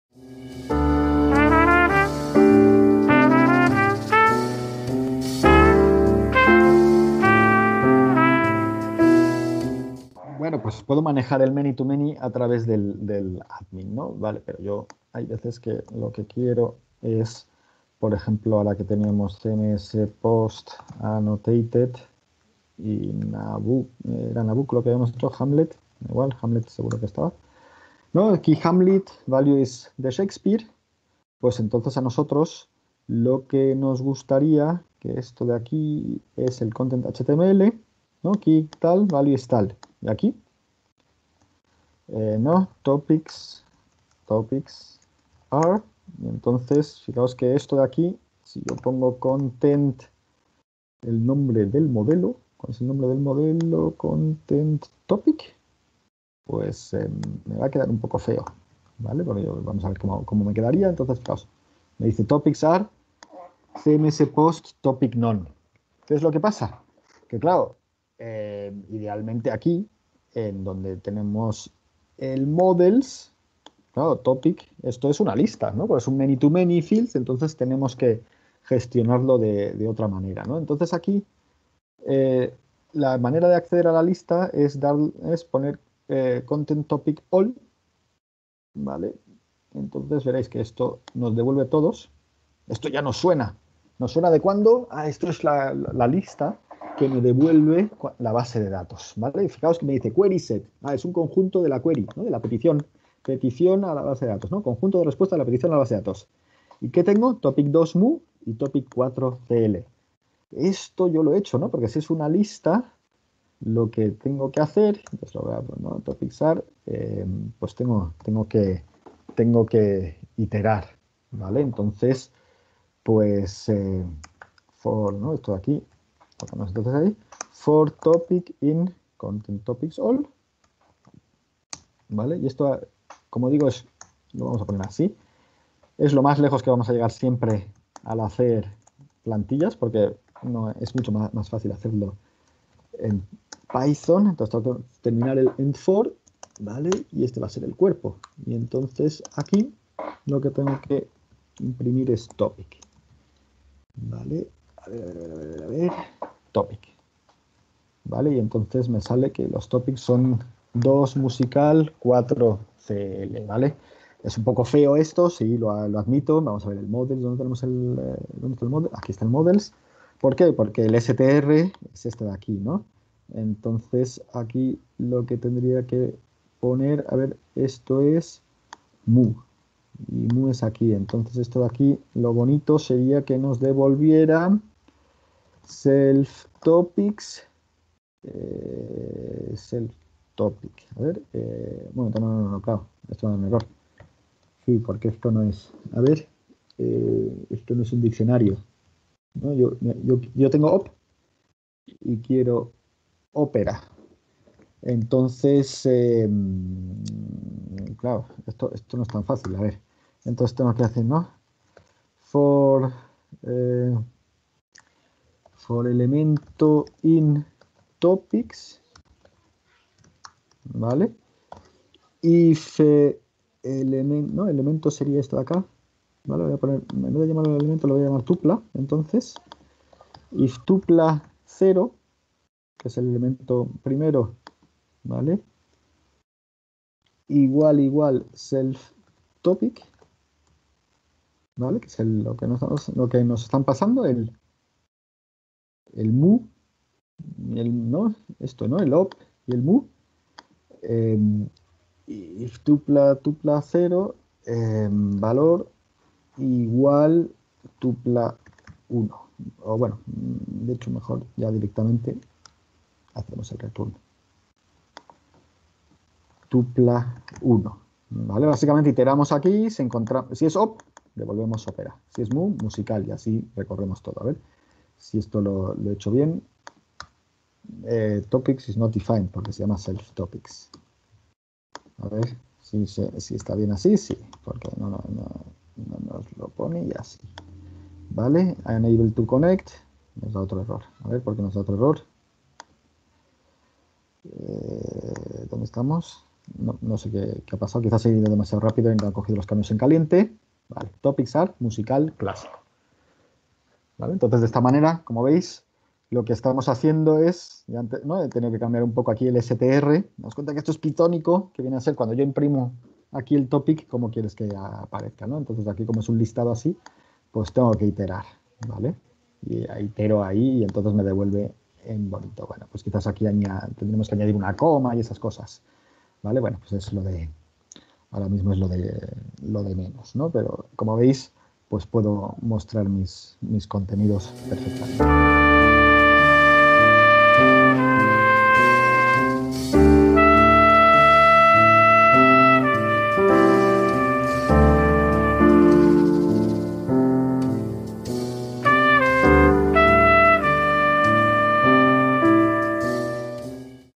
Django. Relaciones entre modelos (3). Uso en plantillas
Los campos de un modelo Django, las referencias en él a otros modelos, pueden referenciarse directamente en las plantillas. Veamos cómo.